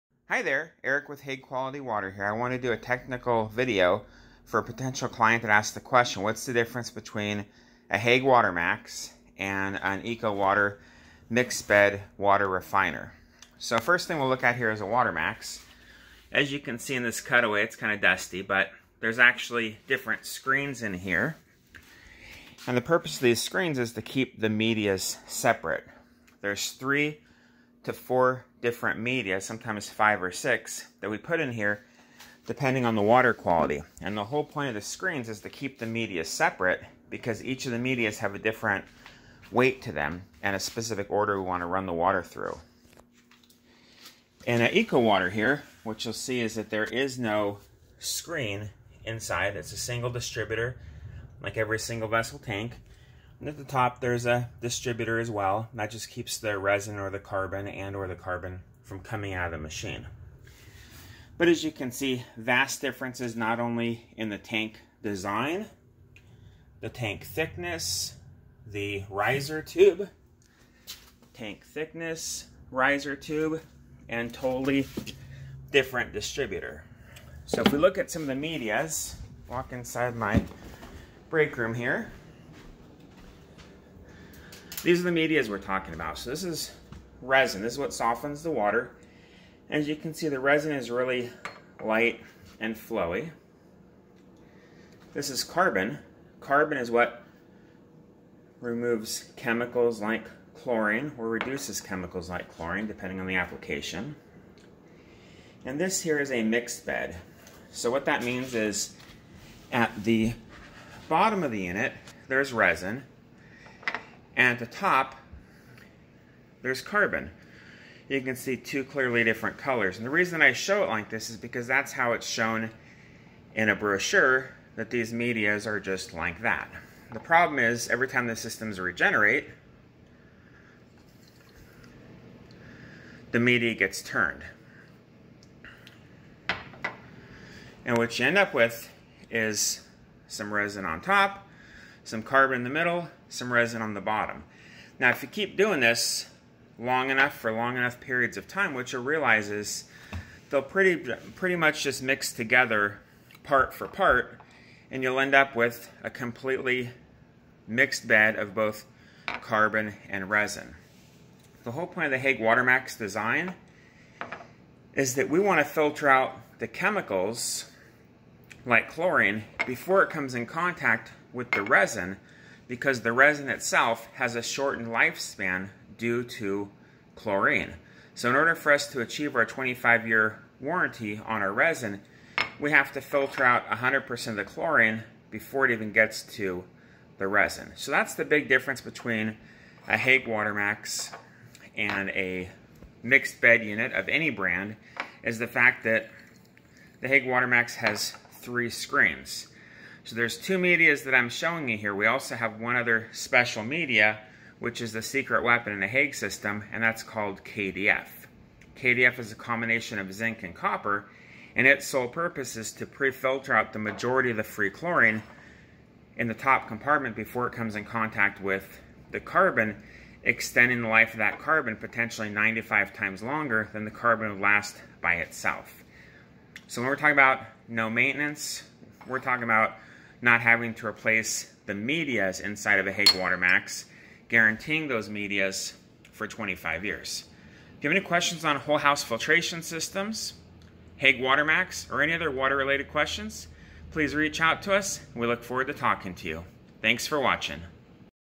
Hague Watermax comparison Ecowater
Single vessel tank versus multi compartment tank. Why it's not the best design to mix media.